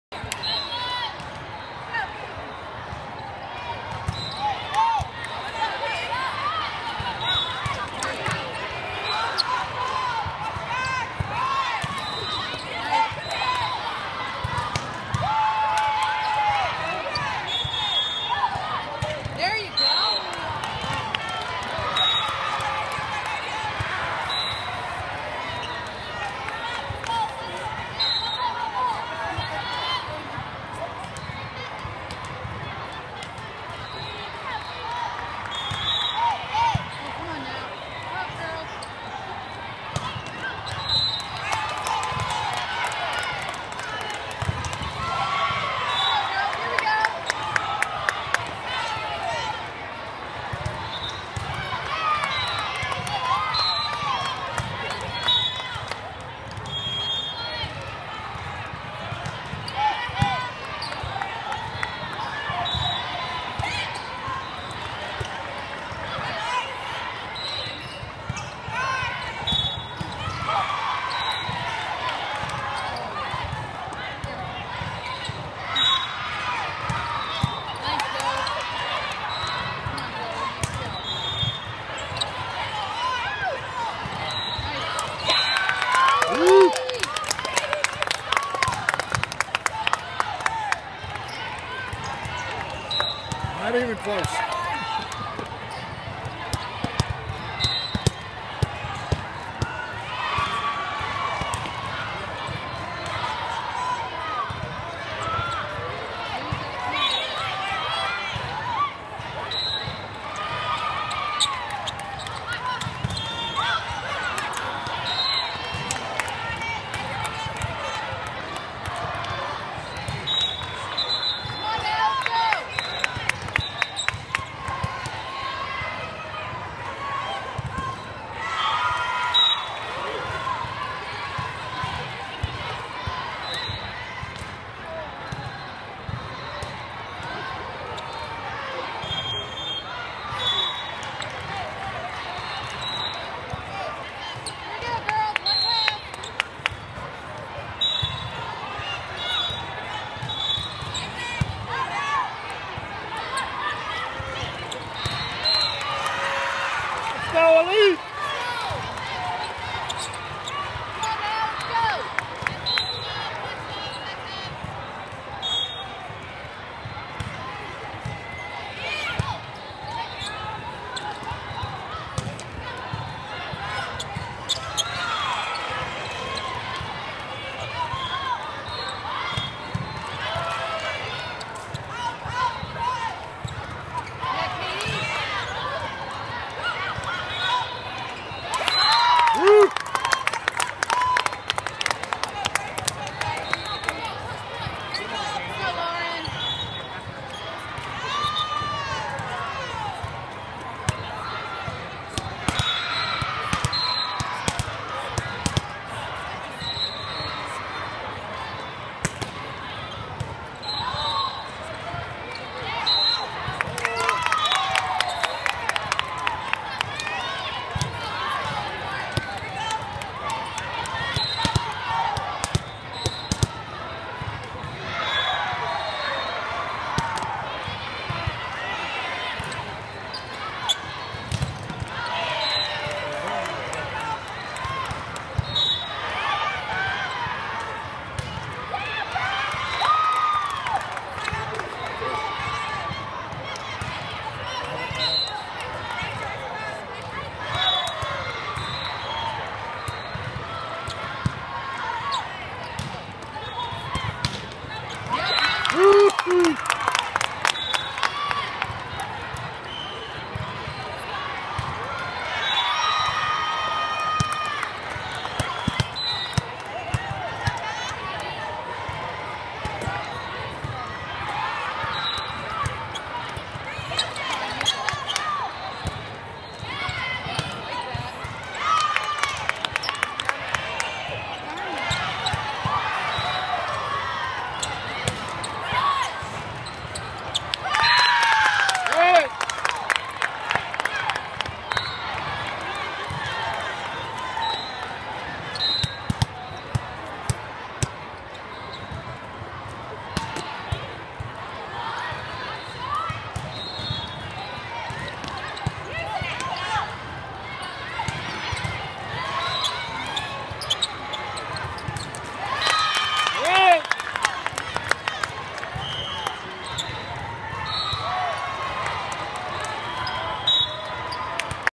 Central Zone Invitational
Silver Bracket Play, 2nd Match (vs NKYVB Tsunami)
Video #2